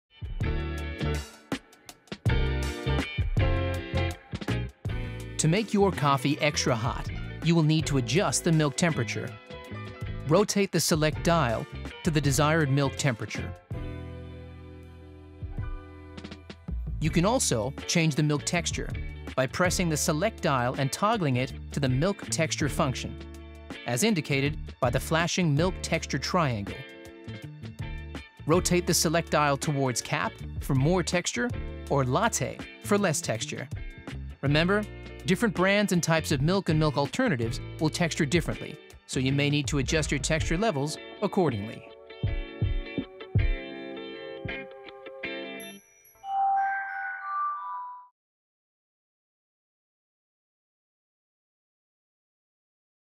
The Oracle™ | How to adjust the milk temperature and its texture like a pro | Breville NZ
Want to recreate the same smooth and creamy texture you get at your local coffee shop? Here’s how.

Texturing milk can be daunting and challenging even for the experienced coffee maker. To help you make delicious and velvety textured milk at home, we’ve created a beginner’s guide to adjusting the milk temperature and texture on your Oracle™ espresso machine.

How to modify the milk temperature on the Oracle™:
1. Press the ‘Select’ dial to adjust the milk temperature function. The temperature will flash on the LCD screen.
2. Rotate the ‘Select’ dial to the desired milk temperature.

How to adjust the milk texture on the Oracle™:
1. Press the ‘Select’ dial and toggle to the ‘Milk texture’ function. This will be indicated by the flashing milk texture triangle.
2. Rotate the ‘Select’ dial towards ‘Capp’ for more texture or ‘Latte’ for less texture.

Remember that different brands of milk and milk alternatives will texture differently, so you may need to adjust your texture levels accordingly. Find out how achieve the perfect milk texture to suit your taste buds
Want to change the espresso shot functionalities on your machine? Here’s our tutorial on how to adjust the shot volume, duration and temperature